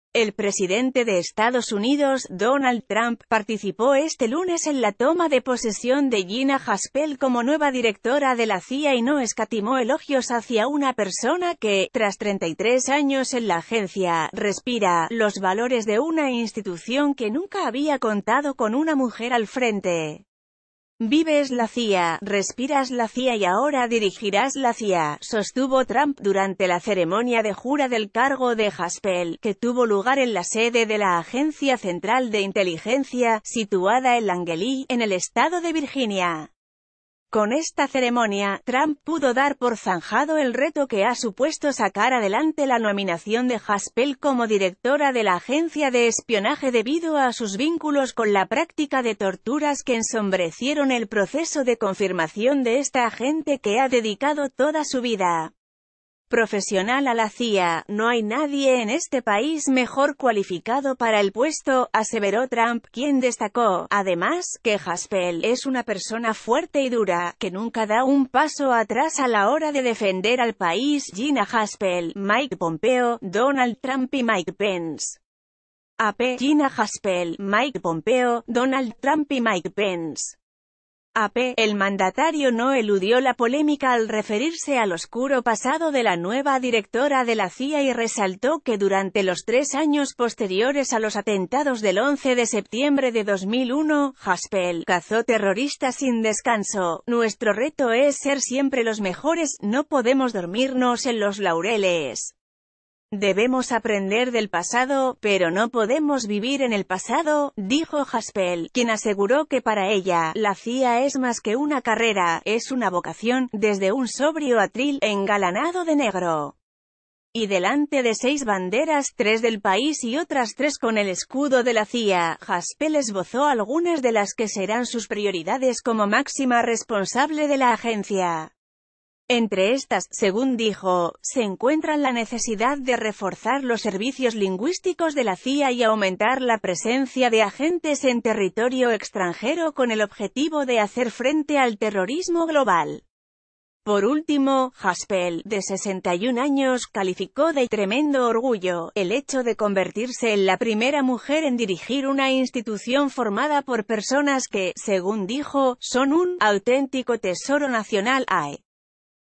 Gina Haspel jura como nueva directora de la CIA
El presidente de Estados Unidos, Donald Trump, participó este lunes en la toma de posesión de Gina Haspel como nueva directora de la CIA y no escatimó elogios hacia una persona que, tras 33 años en la agencia, “respira” los valores de una institución que nunca había contado con una mujer al frente. ...